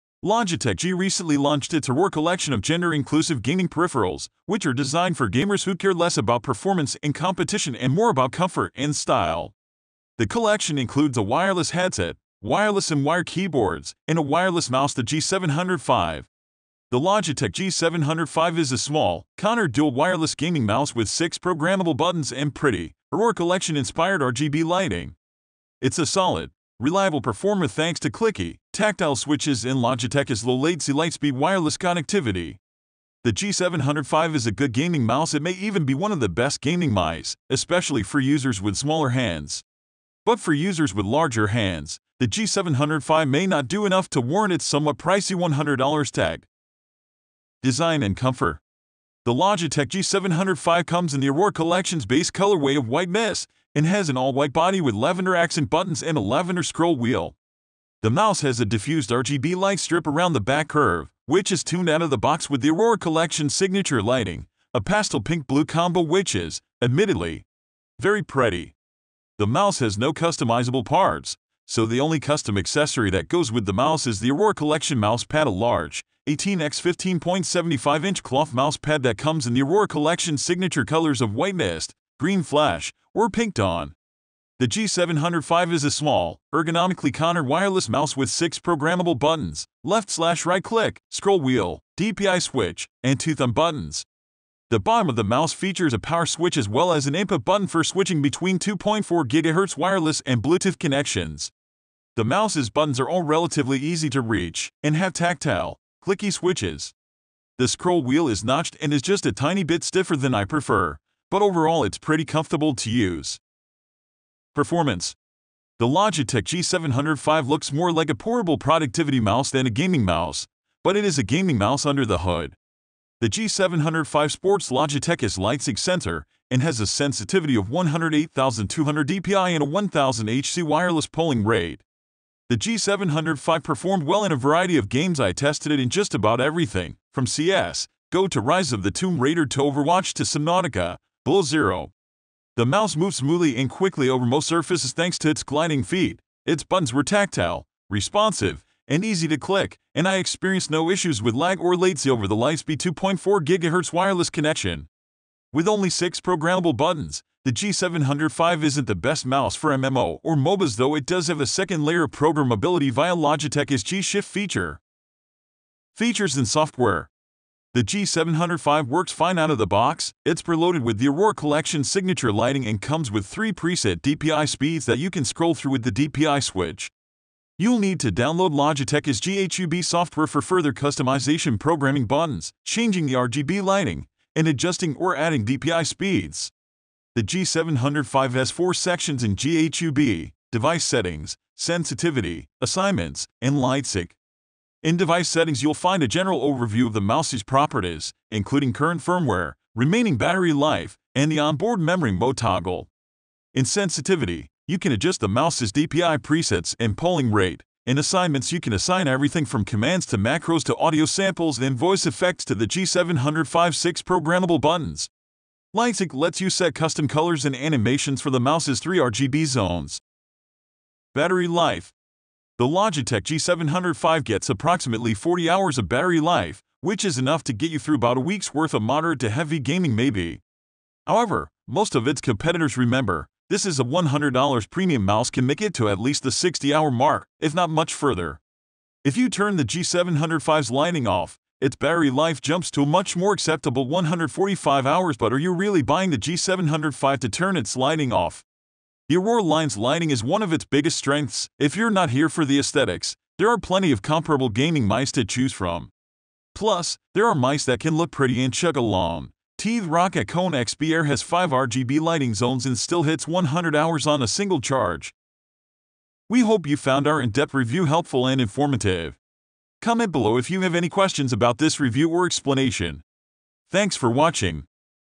Logitech G705 Review 🖱️ Cute, Clicky, Contoured
Get From Here:

Logitech G recently launched its Aurora Collection of “gender-inclusive” gaming peripherals, which are designed for gamers who care less about performance and competition and more about comfort and style. The collection includes a wireless headset, wireless and wired keyboards, and a wireless mouse — the G705.

The Logitech G705 is a small, contoured dual-wireless gaming mouse with six programmable buttons and pretty, Aurora Collection-inspired RGB lighting. It’s a solid, reliable performer thanks to clicky, tactile switches and Logitech’s low-latency “Lightspeed” wireless connectivity. The G705 is a good gaming mouse — it may even be one of the best gaming mice, especially for users with smaller hands. But for users with larger hands, the G705 may not do enough to warrant its somewhat pricey $100 tag.